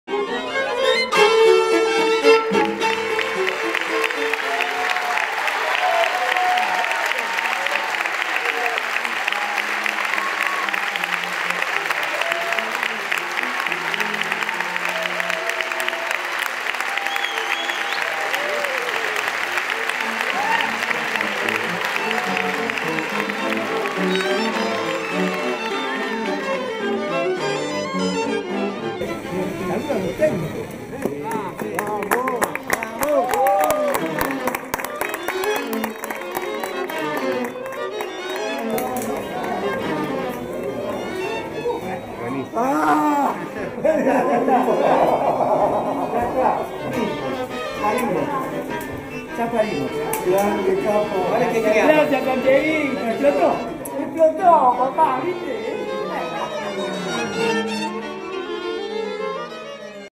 LA FARSA DE LOS AUSENTES - Estreno - Teatro San MArtín
Estreno de “LA FARSA DE LOS AUSENTES”
Sala Martín Coronado con la obra basada en “El desierto entra en la ciudad” de Roberto Arlt, dirigida por Pompeyo Audivert.